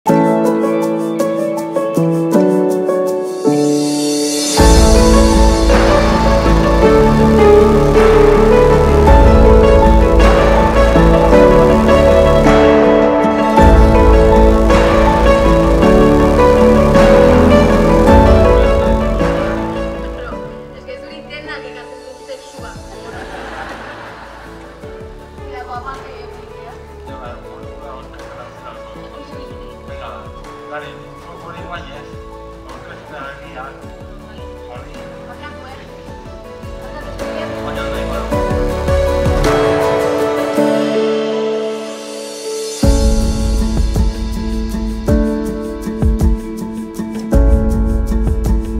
Sexberdinak antzezlana URDINDUZ-en
Gogoratzen Urdinduz egitasmoaren barruan Sexberdinak antzerkia izan genuela Azkoitian? Zenbat barre egin genuen… Hona hemen laburpen bideotxoa.
Mila esker Azkoitiko Udalari sarrerekin bildutakoa doantzan emateagatik.